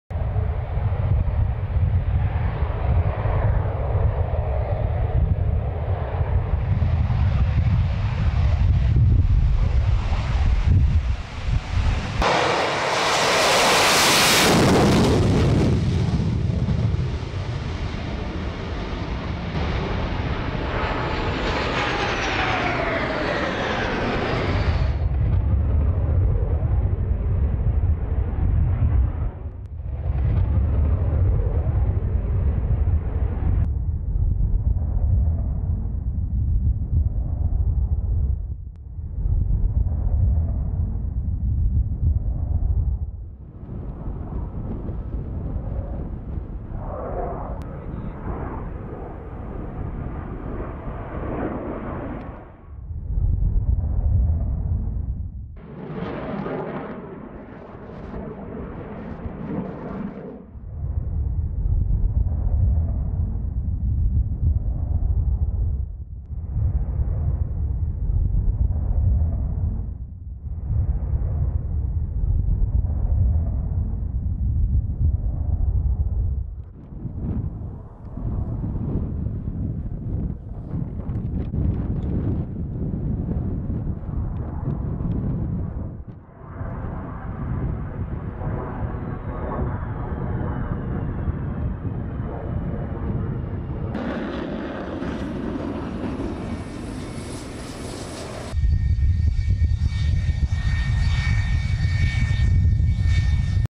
Vuelo del OJOTNIK acompañado por un caza SU-30SM
RUSIA

Vídeo del primer vuelo del dron ruso Ojotnik acompañado de un Su-30SM

El novedoso aparato no tripulado ruso Ojotnik realizó su primer vuelo junto a un avión de combate. El vídeo correspondiente apareció en el canal oficial de YouTube del Ministerio de Defensa de Rusia.

En la grabación se ve cómo el dron despega del aeródromo. Durante el vuelo va acompañado por el caza Su-30SM.